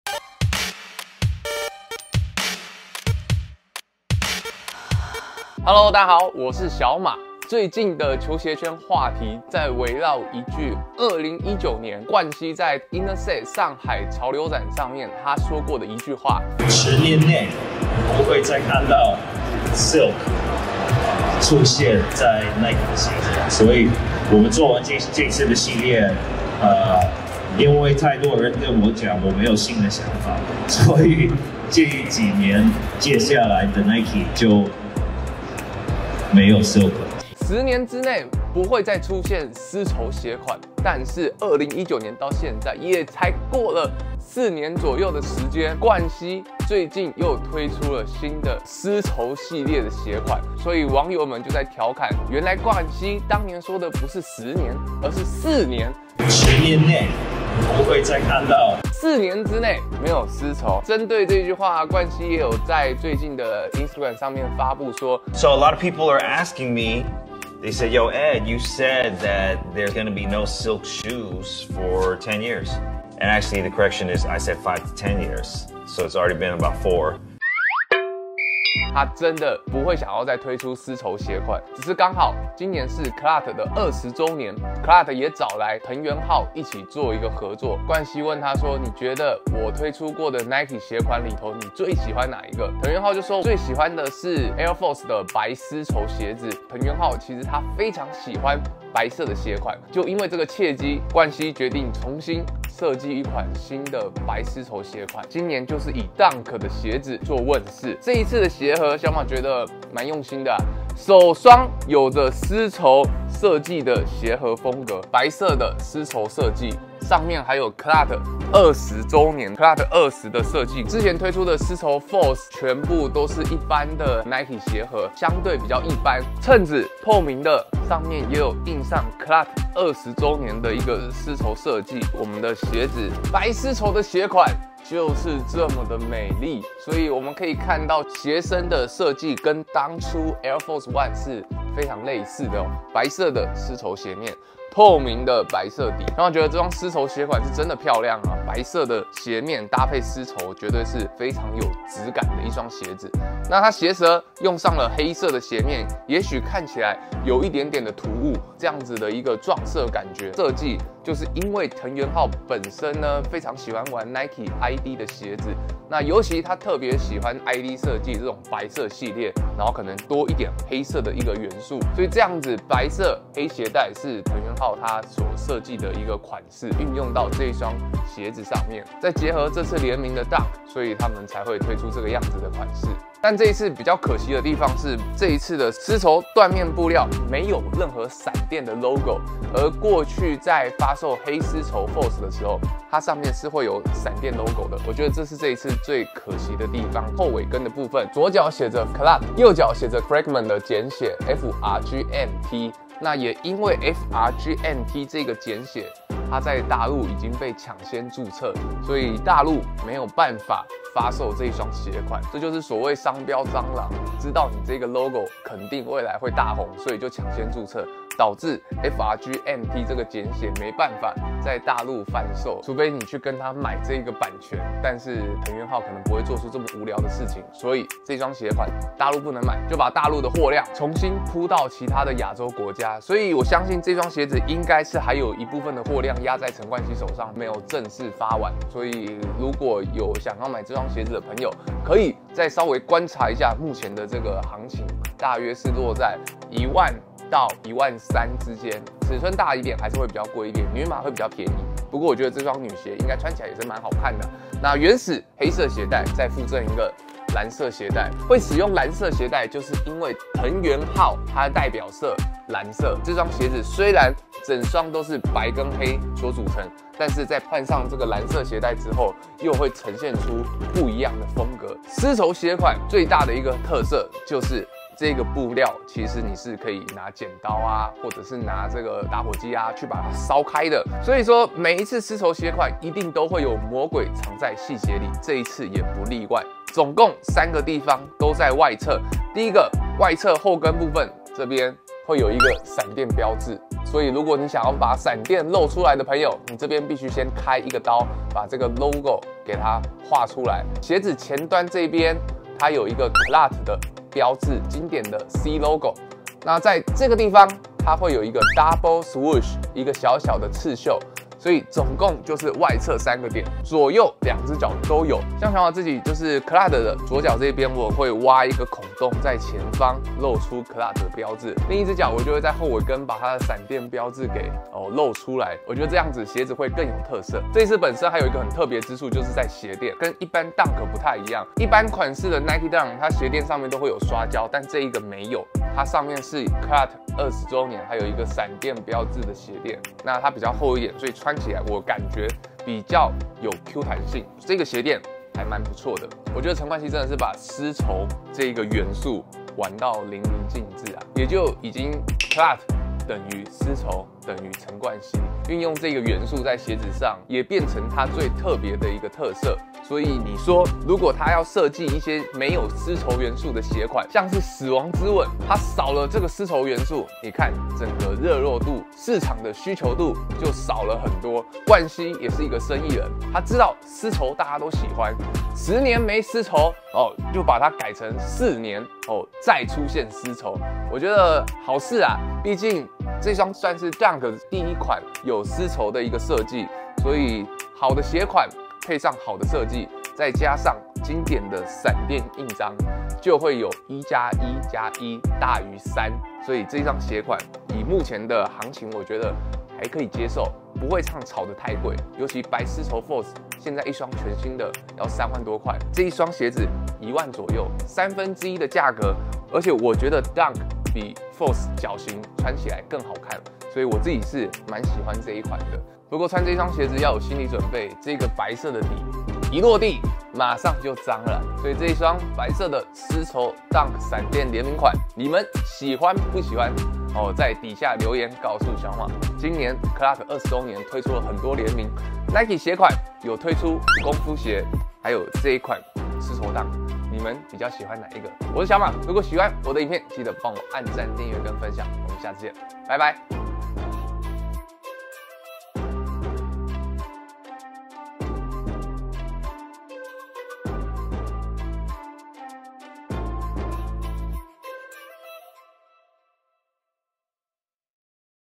重磅?! CLOT 三方聯名閃電白絲綢DUNK 該入手嗎？｜小馬XiaoMa
今天來給大家開箱分享
CLOT X NIKE DUNK X FRAGMENT
原本在2019年時說10年內沒有絲綢的陳冠希
在2023年又找來藤原浩一起推出這雙
以DUNK為鞋型的白絲綢鞋款
各位潔白的鞋身 是否也想入手了呢？
若有廠商想贊助我們,或合作影片可以透過下面連結聯絡